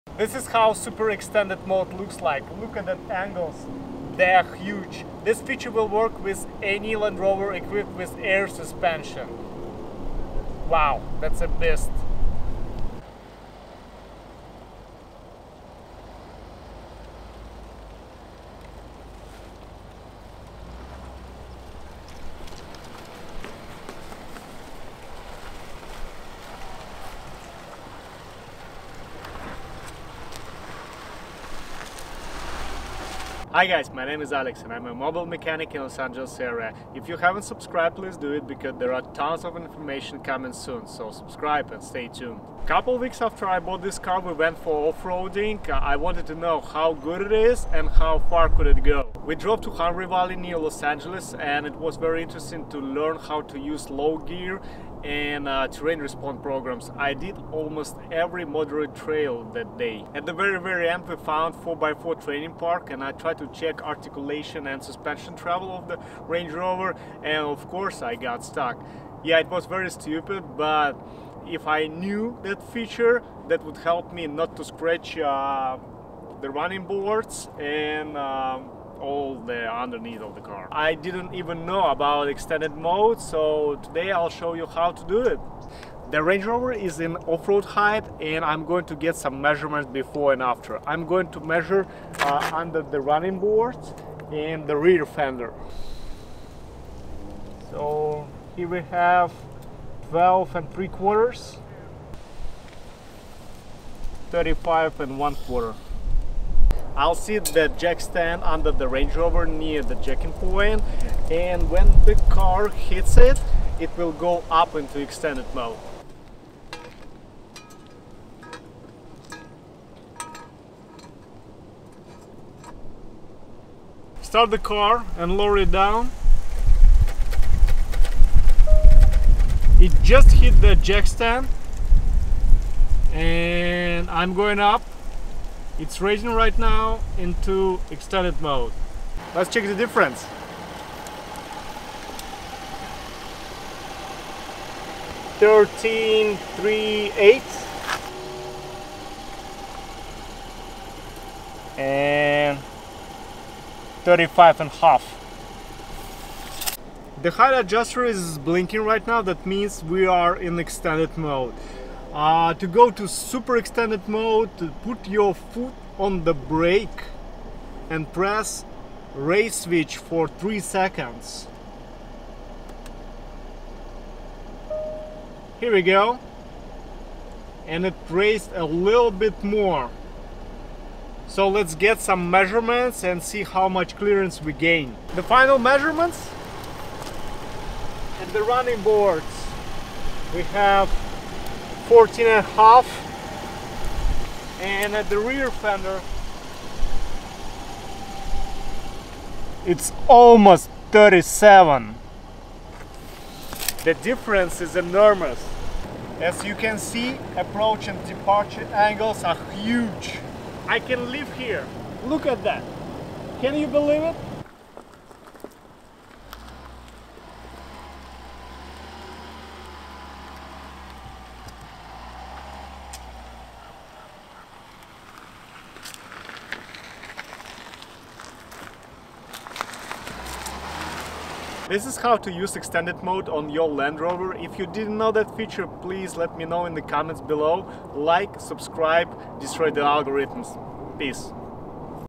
Land Rover Super Extended Mode - Extra Lift You Didn't Know About
Ruinning boards😂😂😂 Sorry!

Hi! This is a step by step guide on how to put your Land Rover into extended mode and then super extended mode. It can be used in the same way on all Land Rover vehicles with air suspension as well as Defender.

Looking for a European car specialist in Southern California? I can help!